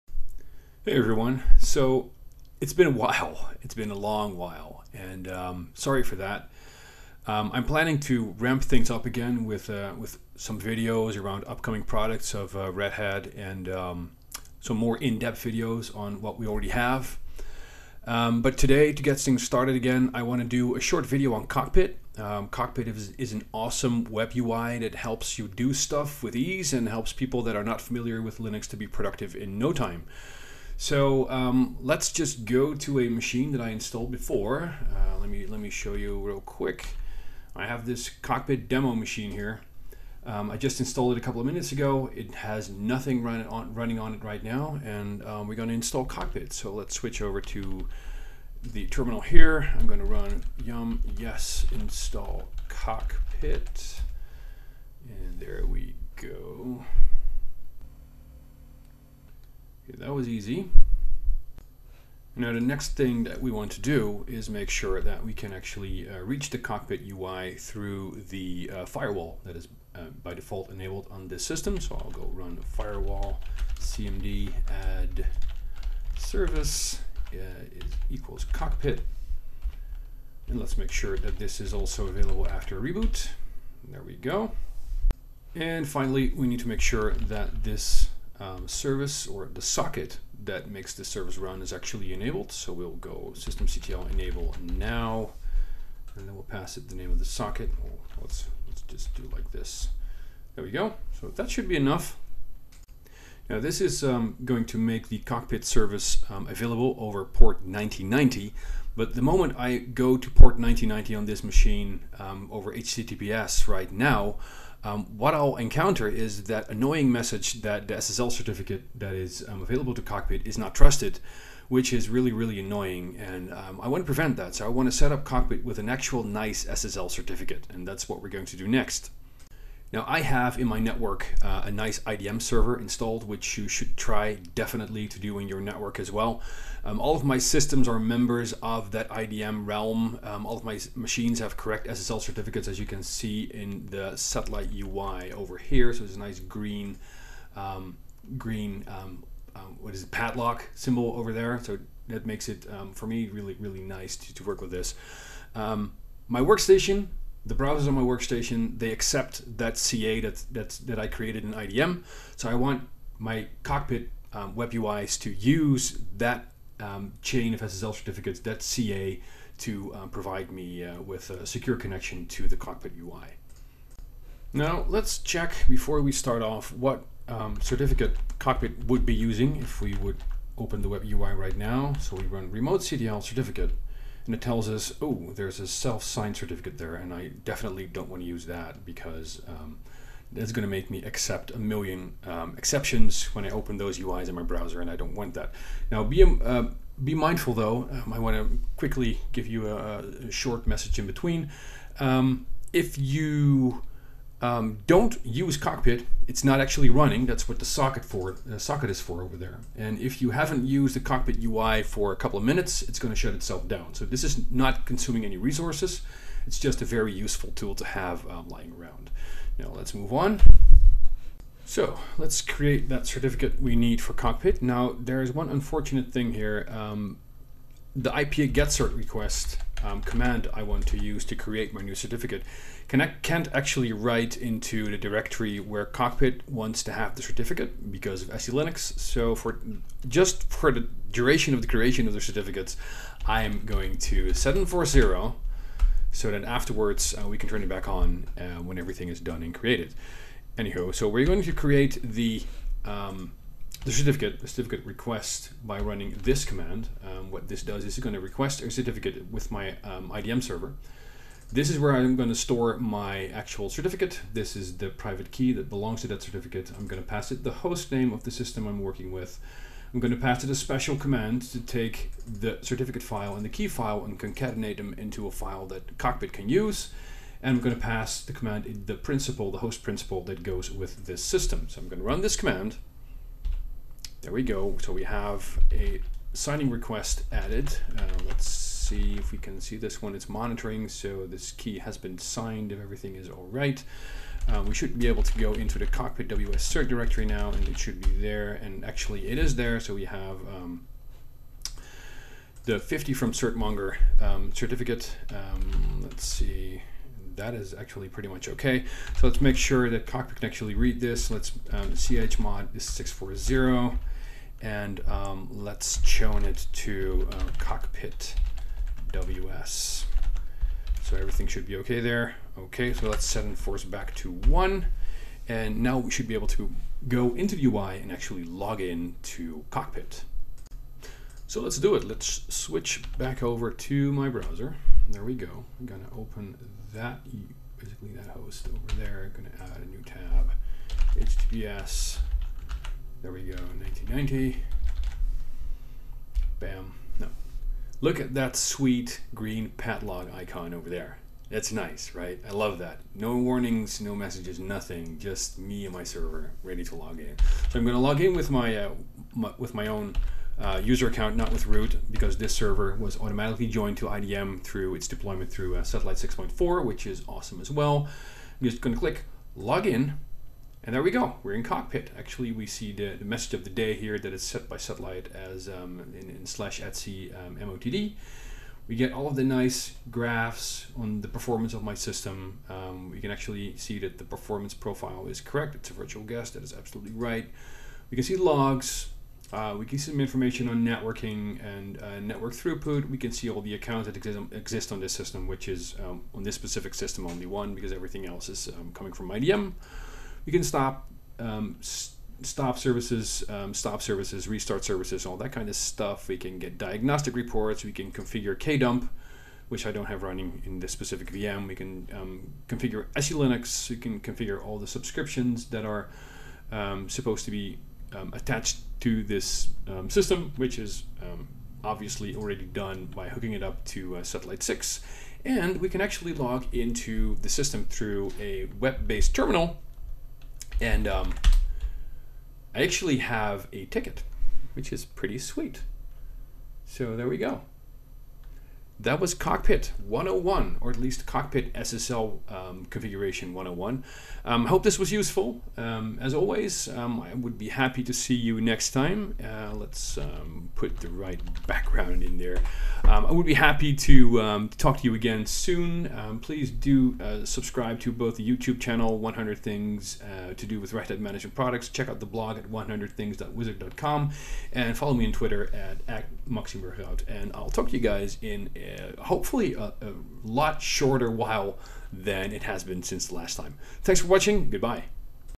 Original: Setting up Cockpit with a trusted SSL certificate (2019)
I did a remake of this video, as my solution didn't work in all cases. Check out the remake here, it's about both RHEL7 and RHEL8

Cockpit is an awesome tool to manage systems with. Even for experienced admins, it has some nice features that come in handy every now and then, like live performance metrics, just to name one.

If you're a little less familiar with Linux, the Web Console as we call our enterprise version of Cockpit in Red Hat Enterprise Linux, make your life a *lot* easier! You can do most common systems administration tasks through a web UI now, like configuring networking, configuring storage and keeping track of system performance. Great!

However, I *hate* seeing my browsers complain about untrusted SSL certificates and having to add an exception for each system that I log into. This video was made to help you prevent that, by showing how to add a trusted SSL certificate to Cockpit on RHEL7 and up.